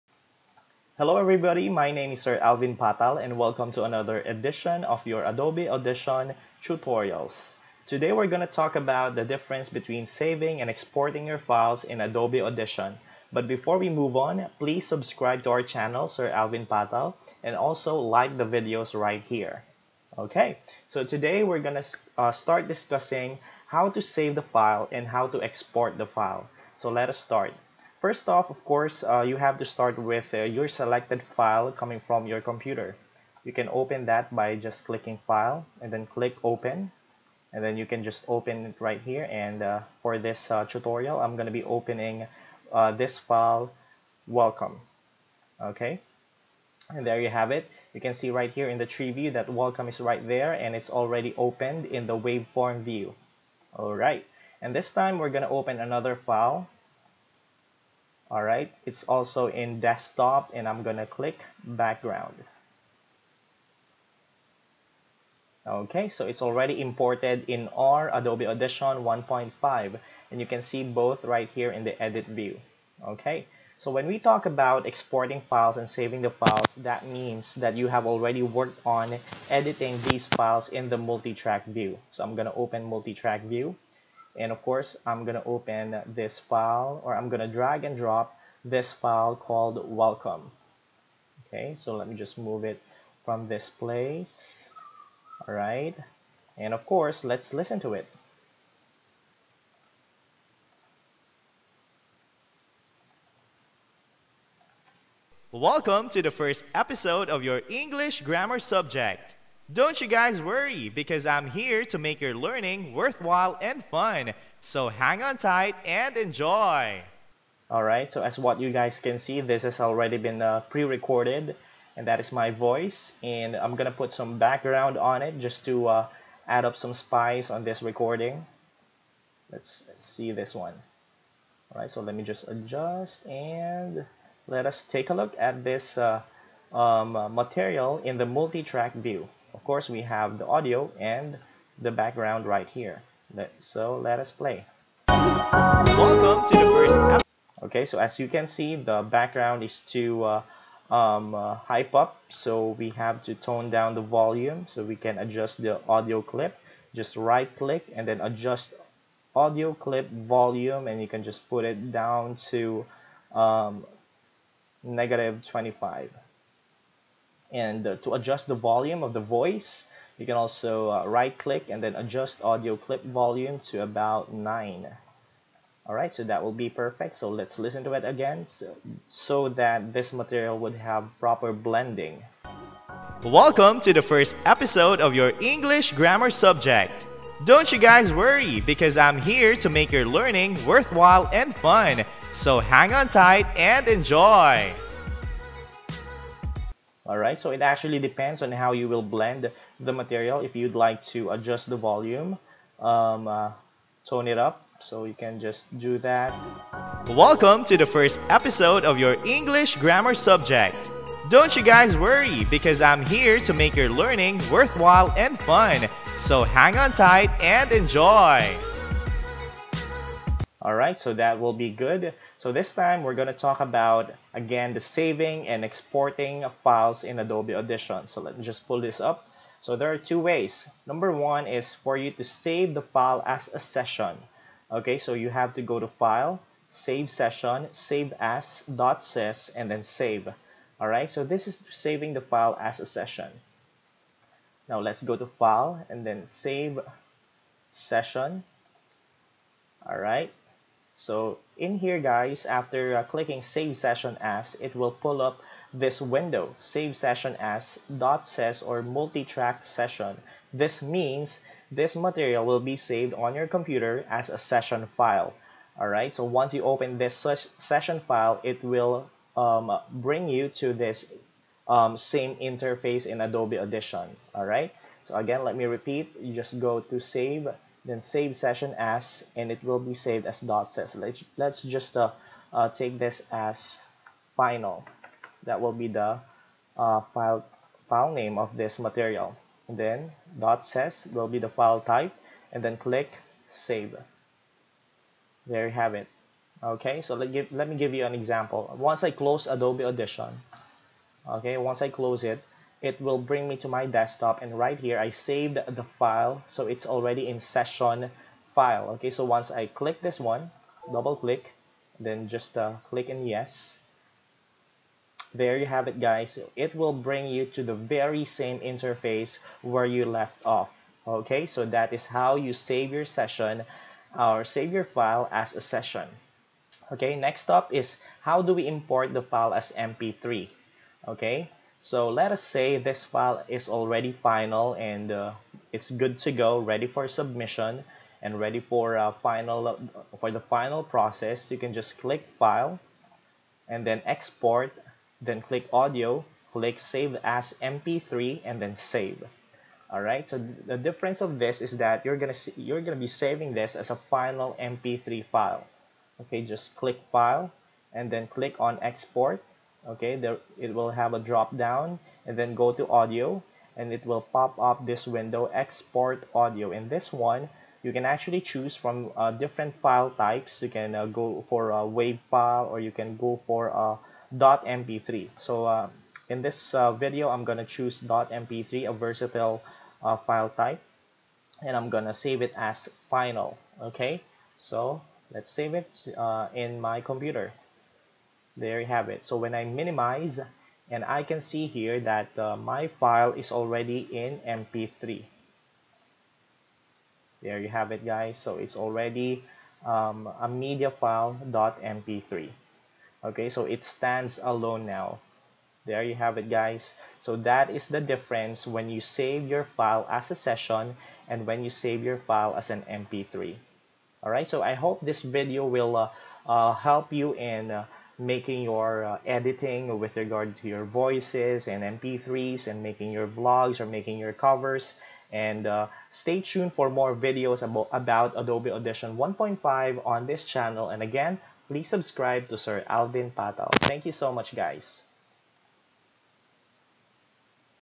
How to save and export files in Adobe Audition 1.5
Want to know how to save the file as an "editable" file and "final" file? Check this video out.